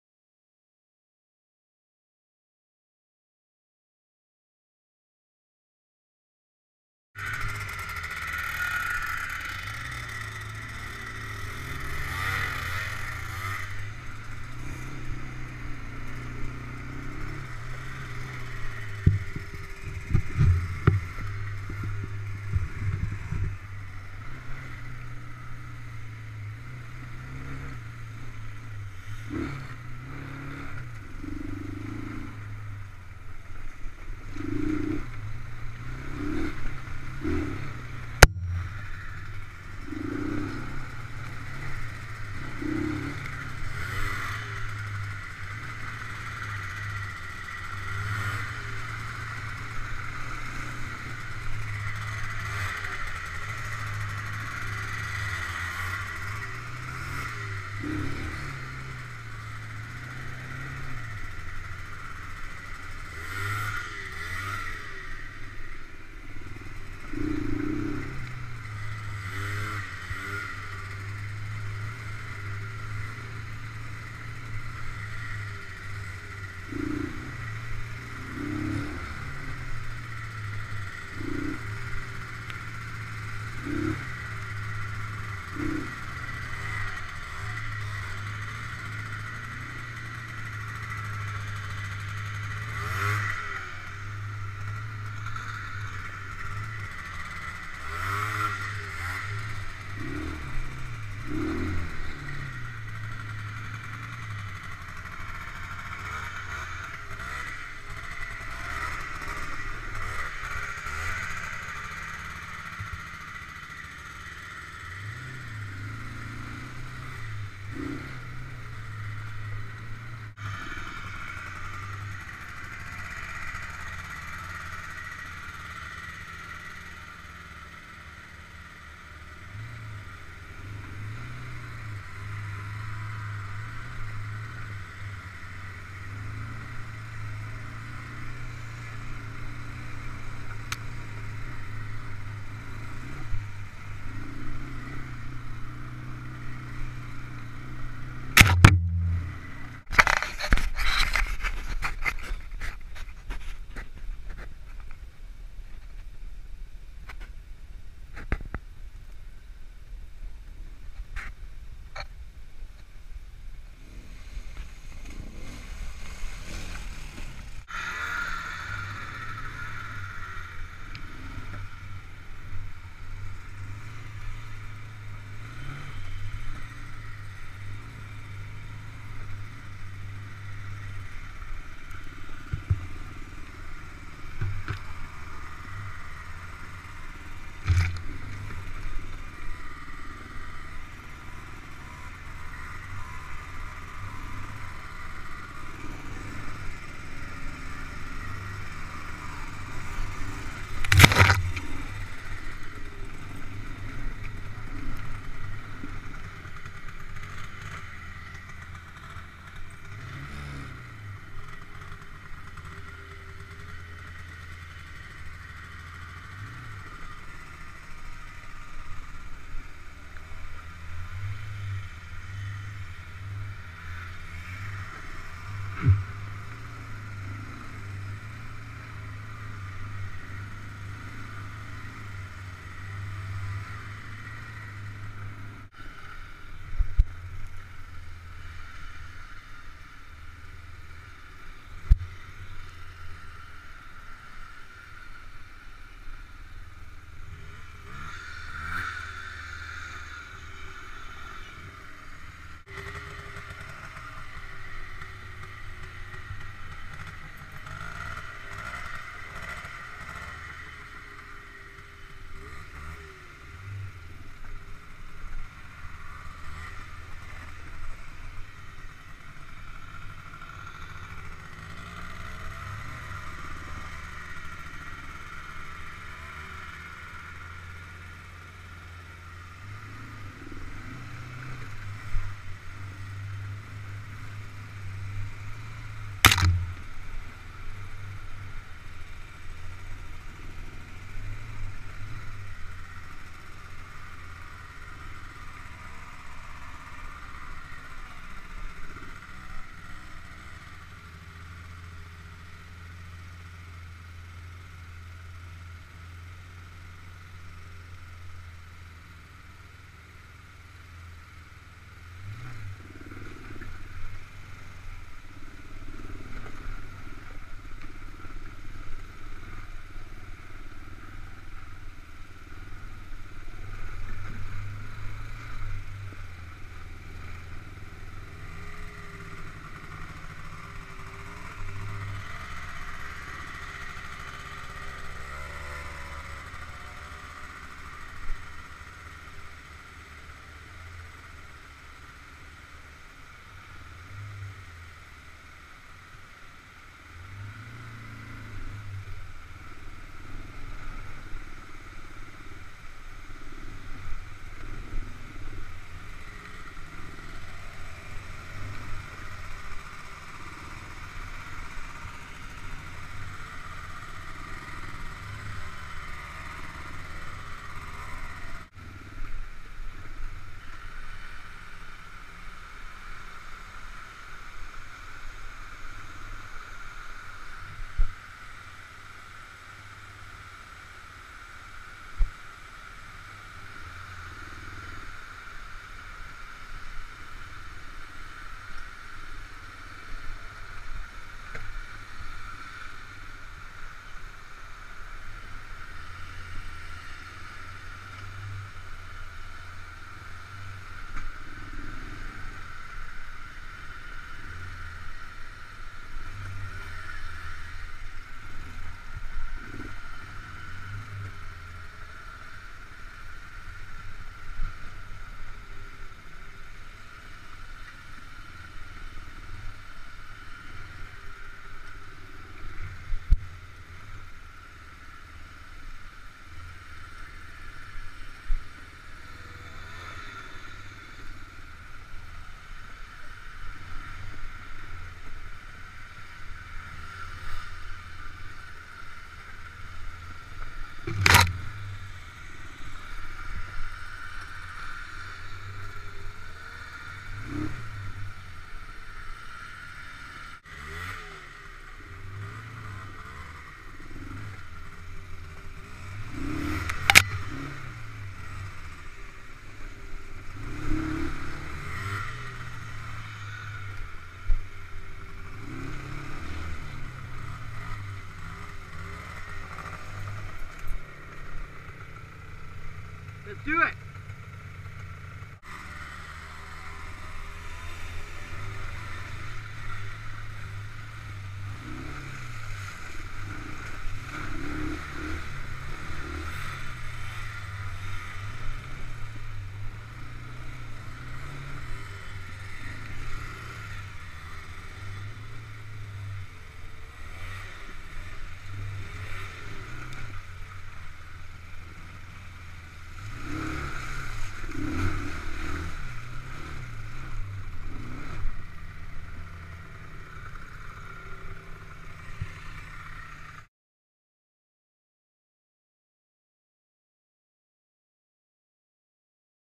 Tamahi creek chilliwack
my dad and i having some fun riding in Chilliwack on may long weekend.